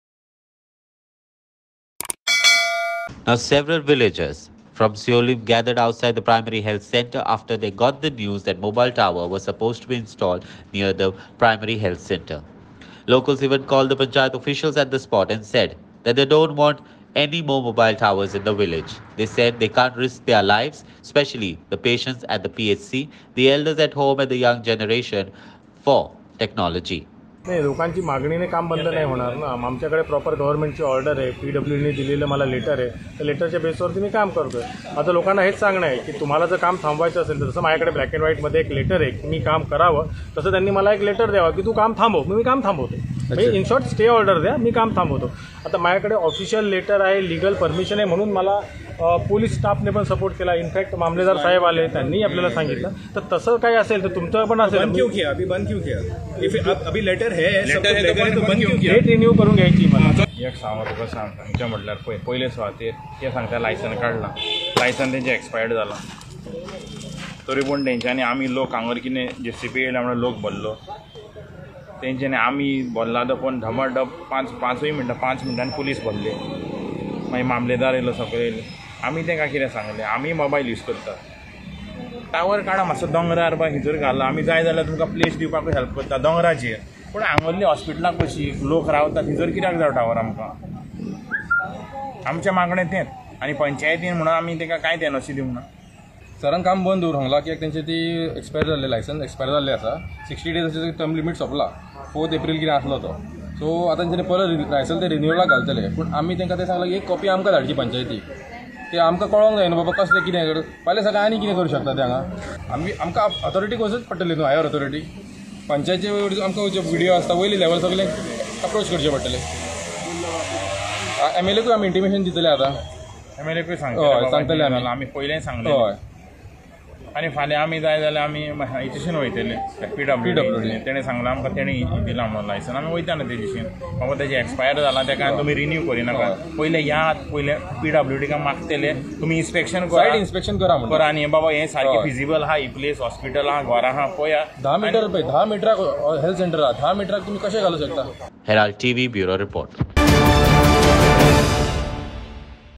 Siol’kar unite against proposed mobile tower near Primary Health Centre
Several villagers from Siolim gathered outside the Primary Health Centre (PHC) after they got the news that mobile tower was supposed to be installed near the PHC. Locals even called the panchayat official to the spot and said that they dont want any more mobile towers in the village. They said they cant risk their lives especially of the patients at PHC, elders at home and the younger generation, for technology.